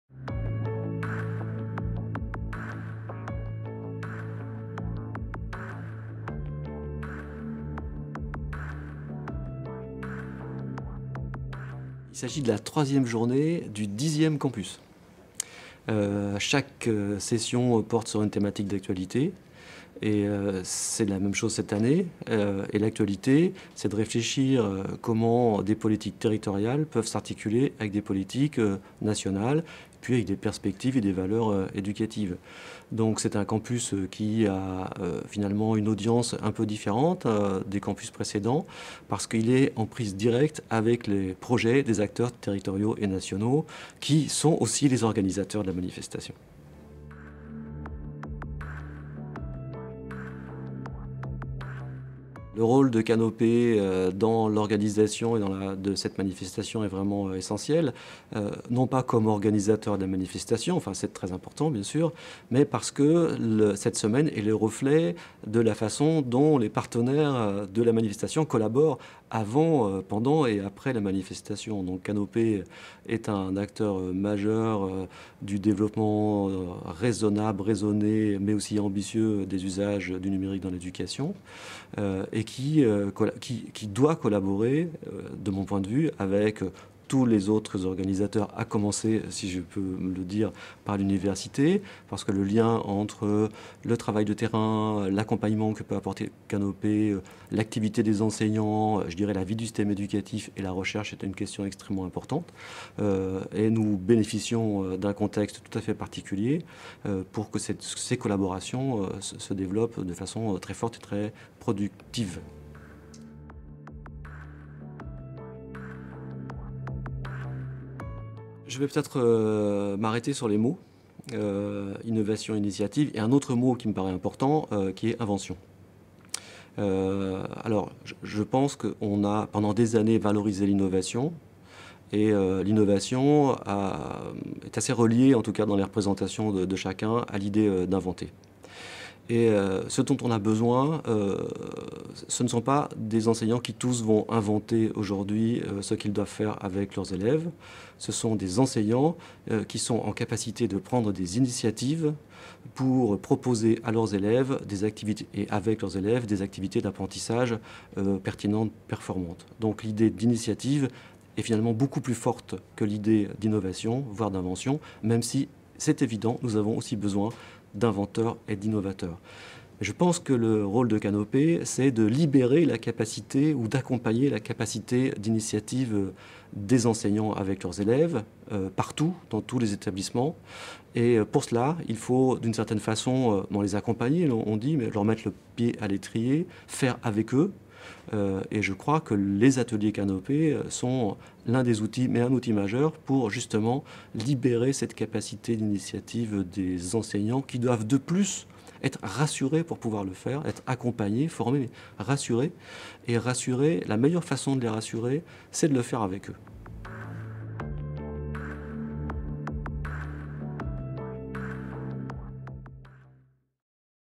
Interview de Jean-François Cerisier - Campus d'été de l'université de Poitiers 2014
Interview de Jean-François Cerisier - vice-président de l'université de Poitiers en charge du numérique et de la documentation
Campus européen d'été de l'université de Poitiers 2014. Le thème de cette édition « Territoires connectés : enjeux et perspectives pour l'éducation et la culture ». Présentation du rôle de Canopé et de ses Ateliers dans le cadre de cette thématique.